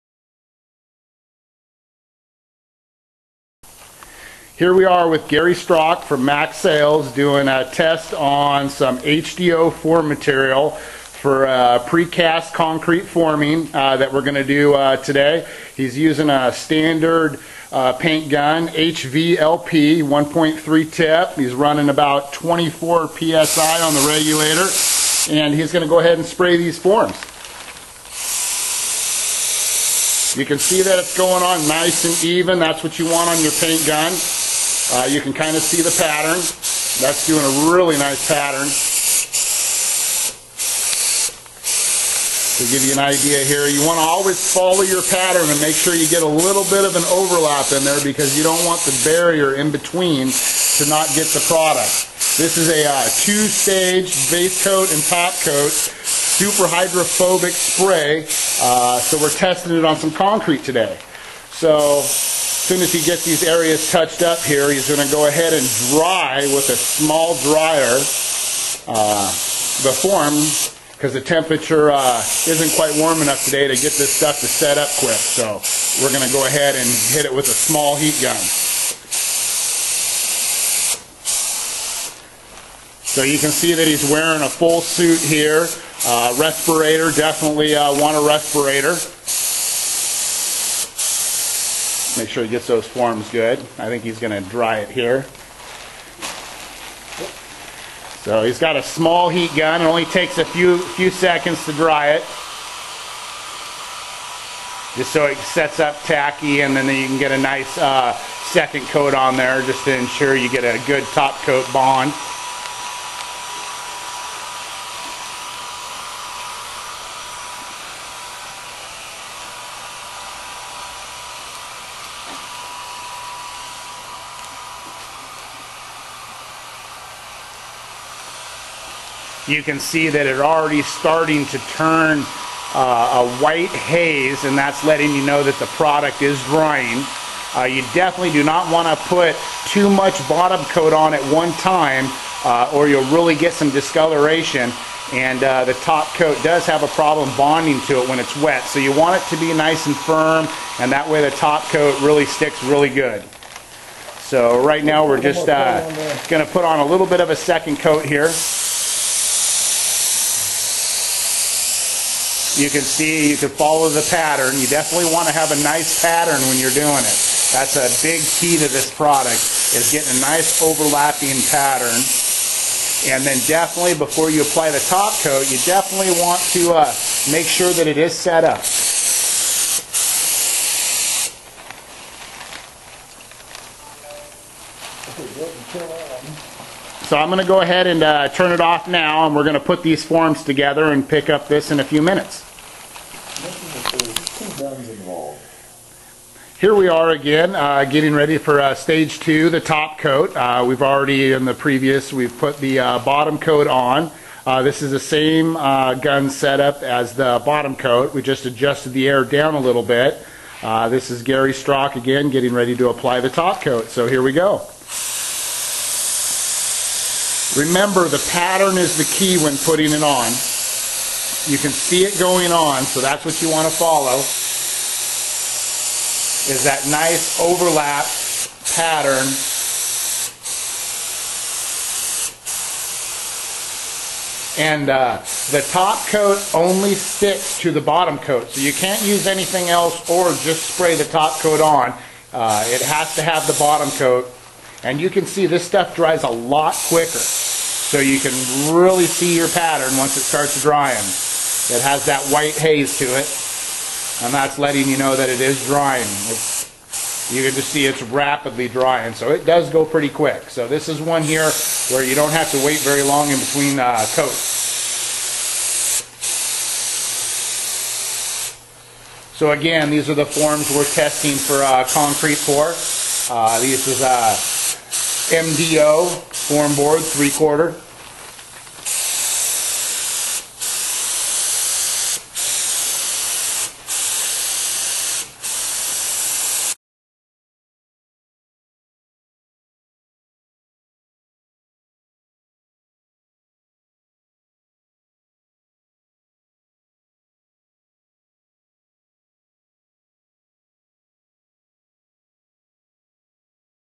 Ultra Ever Dry on Concrete Forms
Ultra Ever Dry being applied to concrete forms for swppp (to prevent seepage of oils from concrete mold into watershed areas). For more information on considerations, tips and performance of Ultra Ever-Dry on concrete forms, and click on ULTRA EVER-DRY LAB.